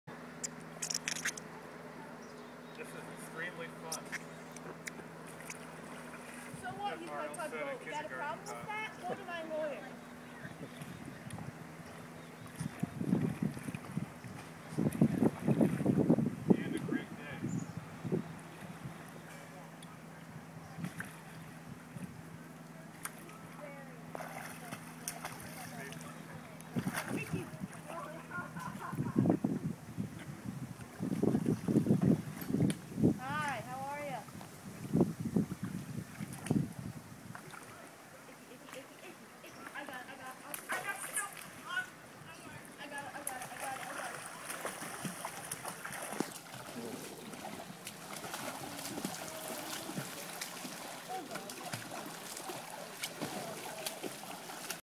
IMG 0235 Water Fun at Camp
Daniel Haverstock and his son Michael took on the challenge of Stand-Up-Paddleboarding at their Aunt Margaret's camp at Higley Flow in Colton, NY. (July 11, 2014)